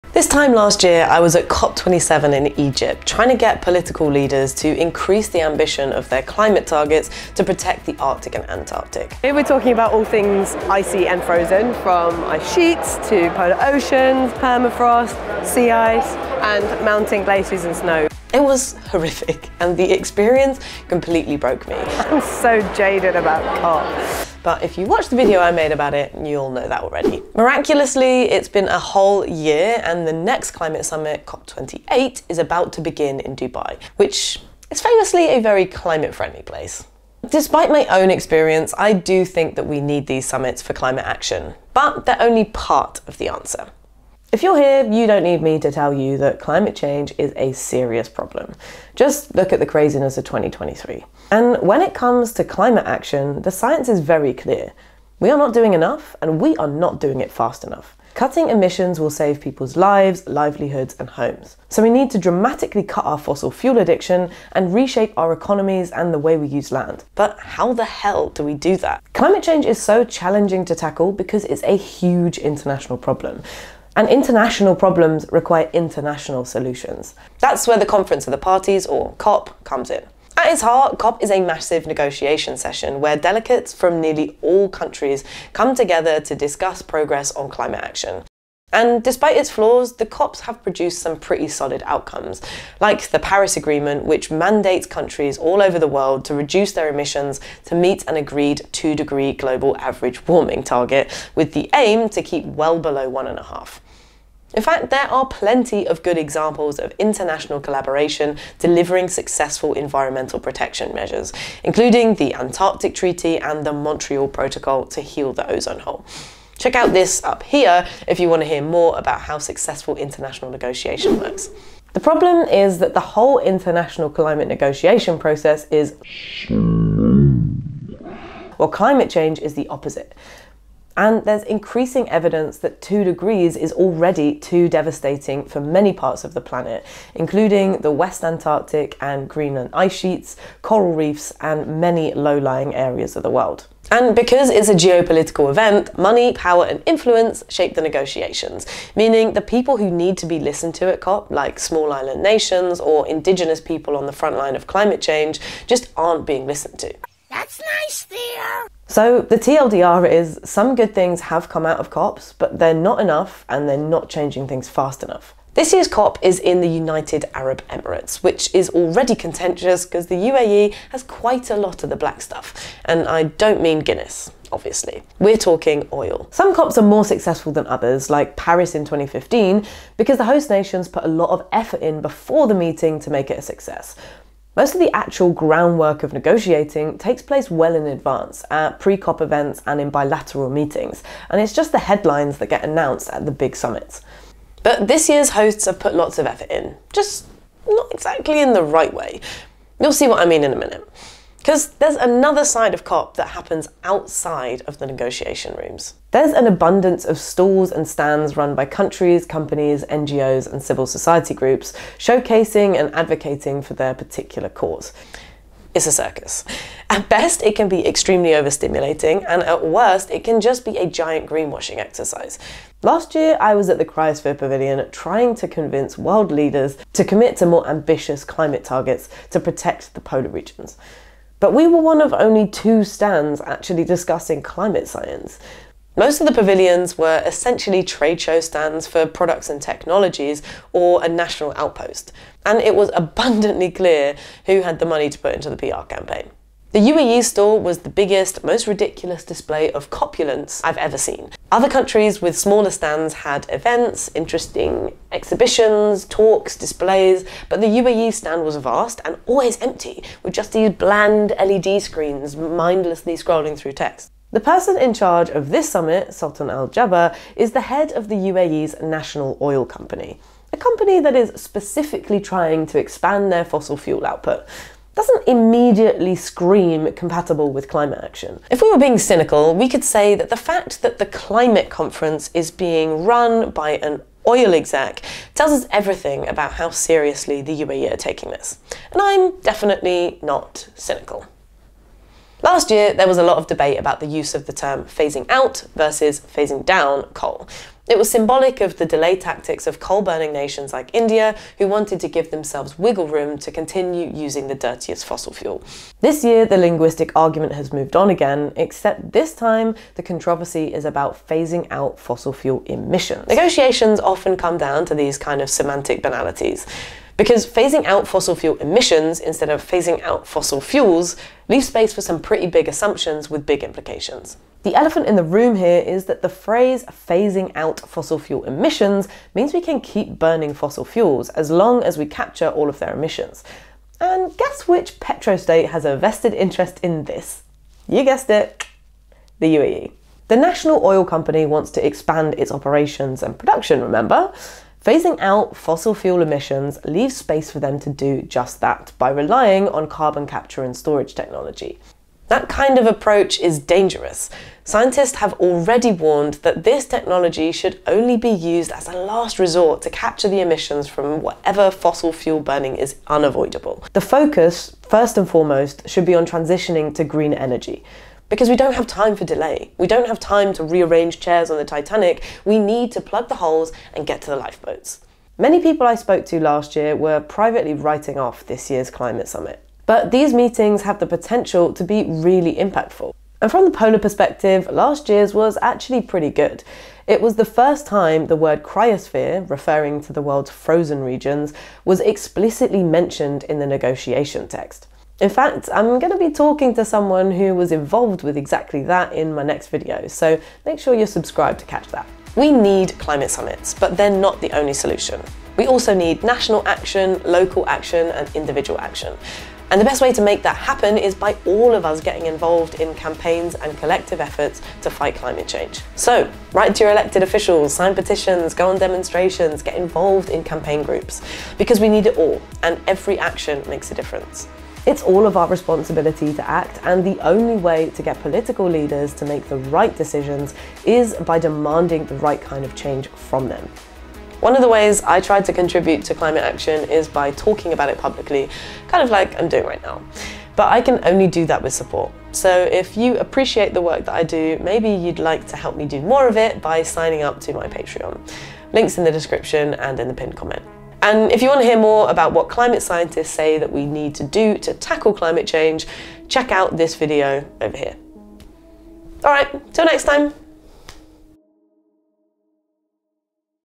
What's the point of climate summits anyway?
I hate COP climate summits. And this year’s is especially contentious (I’m lookin’ at you, UAE). But we still need them, because international problems need international solutions.

COPs are one part of the answer, and everything else – national, local and individual action – is the rest.

While we're on the topic, you should also check out Climate Adam’s fantastic video about COP28

Contents

00:00 Intro
00:43 Why do we need COPs?
01:09 International negotiations
02:45 The circus
04:07 Greenwashing
06:15 Necessary but not sufficient

Subscriptions help me make more (& better!) video content about climate and polar science, and keep these videos independent.

COP image credits:
President of Armenia CC-BY 3.0
President of Bulgaria CC-BY 3.0
Dati Bendo
Arctic Circle